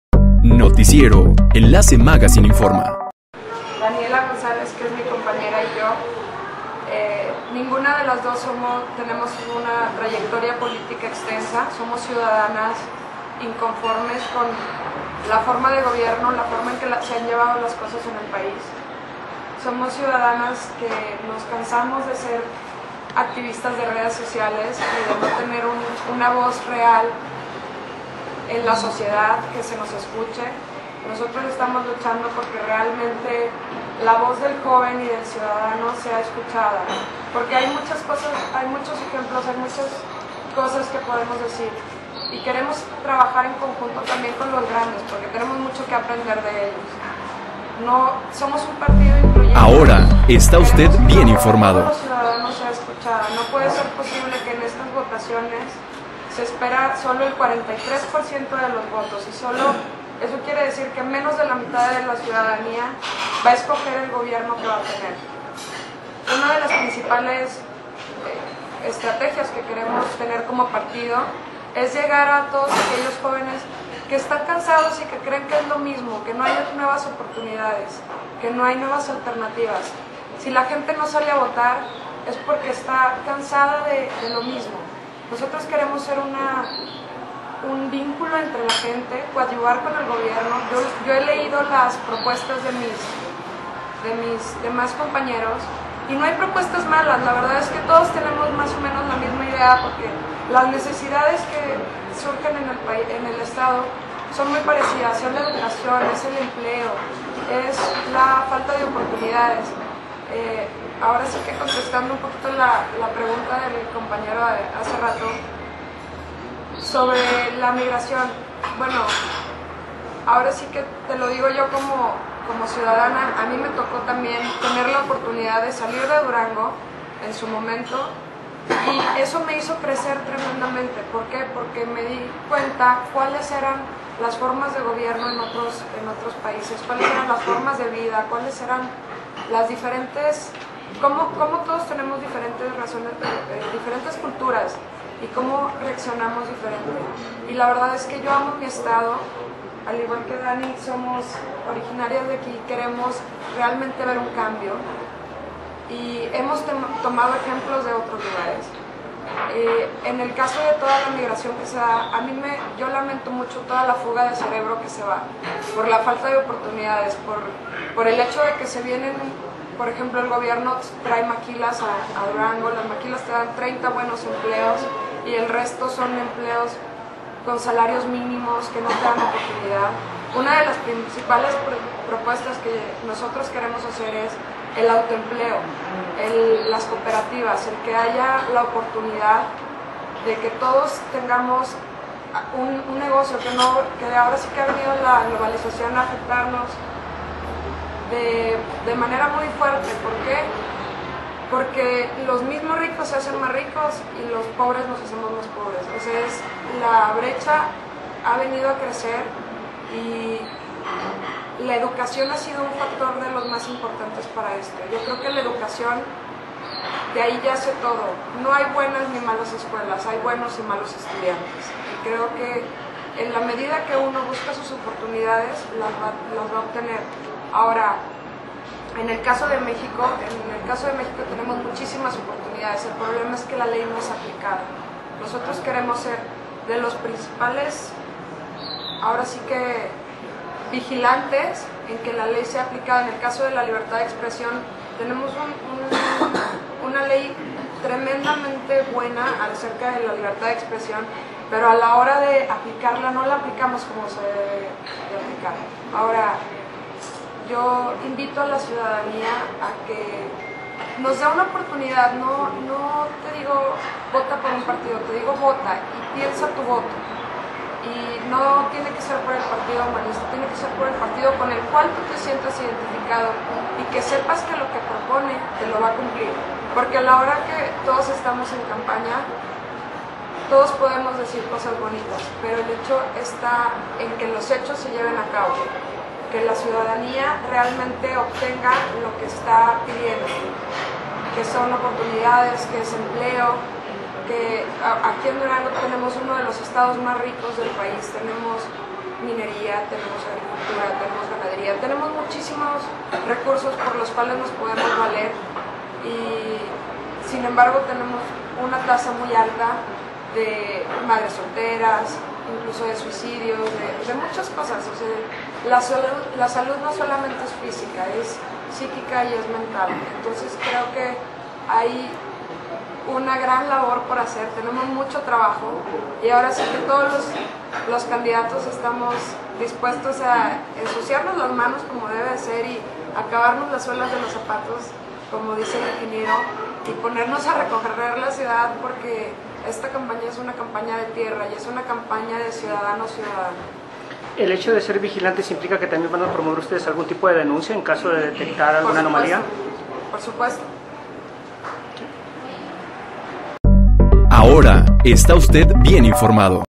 Ante la crisis, el autompleo, opción para profesionistas; Partido Humanista
La situación económica reinante el el país, y la escasa oportunidad laboral para los egresados,  candidatos del Partido Humanista en Durango proponen el autoempleo como una alternativa de solución para  los egresados.